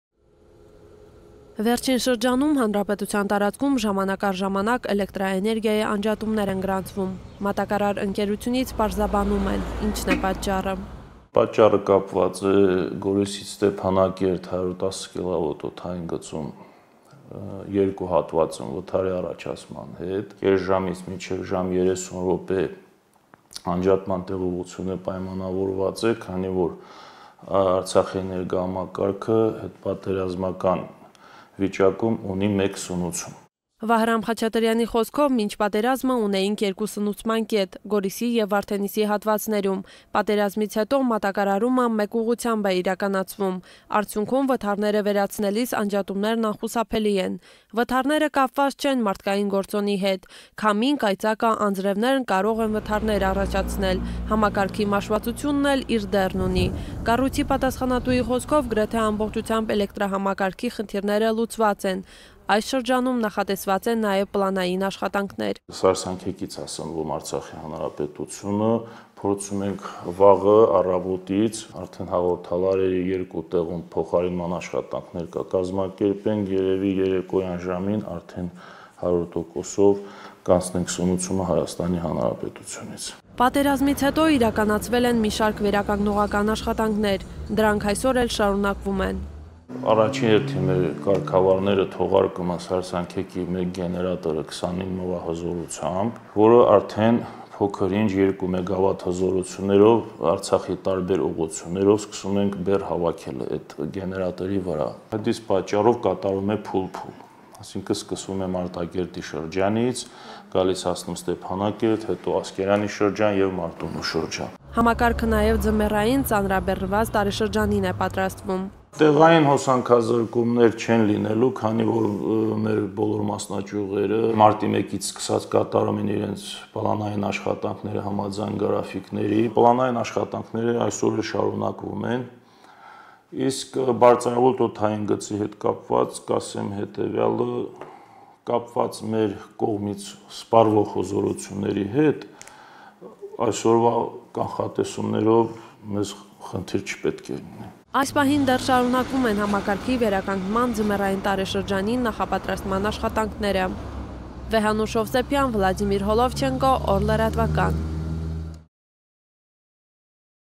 ՀՈՍԱՆՔԱԶՐԿՈՒՄՆԵՐԻ ՄԱՍԻՆ՝ ԱՌԱՋԻՆ ՁԵՌՔԻՑ
Վերջին մի քանի օրվա ընթացքում հաճախակի են դարձել էլեկտրամատակարարման դադարեցումները: Մատակարարն ունի իր հիմնավորումները: Համակարգը կարգավորելու ուղղությամբ աշխատանքները շարունակվում են արդեն ամիսներ շարունակ: Որոշ խափանումներ վերացնելու համար էլ ստիպված են անջատումներ կատարել՝ դեպքերի մեծ մասում պլանավորված, բայց երբեմն էլ՝ իրավիճակից ելնելով: Ընթացիկ աշխատանքներին զուգահեռ՝ էներգոհամակարգը նախապատրաստում են ձմեռային տարեշրջանին: